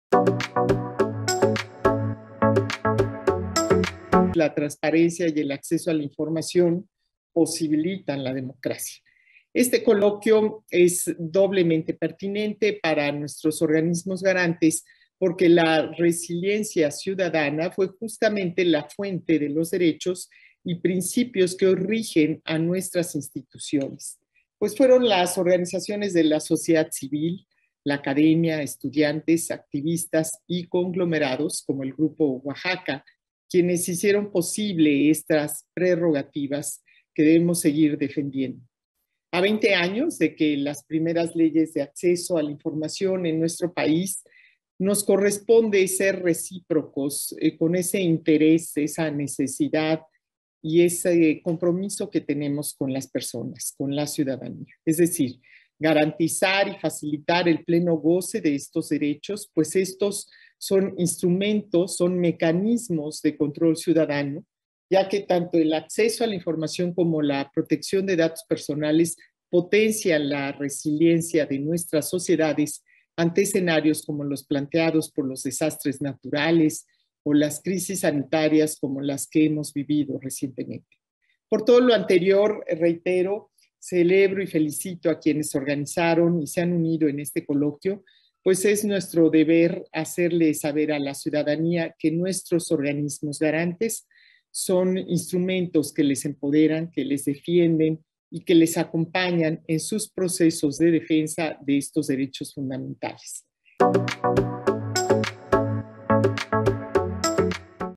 Blanca Lilia Ibarra C. en el 4o Coloquio por una Reconstrucción Abierta - Resiliencia y Democracia
Blanca Lilia Ibarra Cadena, Comisionada Presidenta del INAI, en el 4o Coloquio por una Reconstrucción Abierta - Resiliencia y Democracia